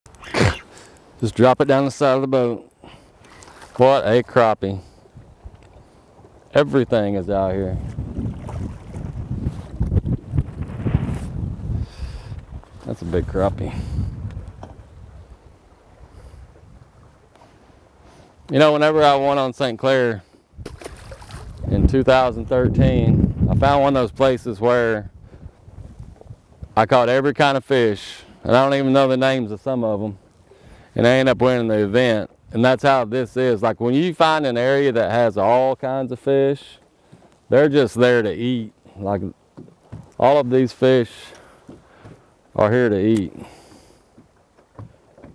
They're here to EAT (Catching multiple species)
They're here to EAT (Catching multiple species) – Sometimes catching a lot of different species in one area isn't a bad thing.

Videography & Edit by Creating Outdoors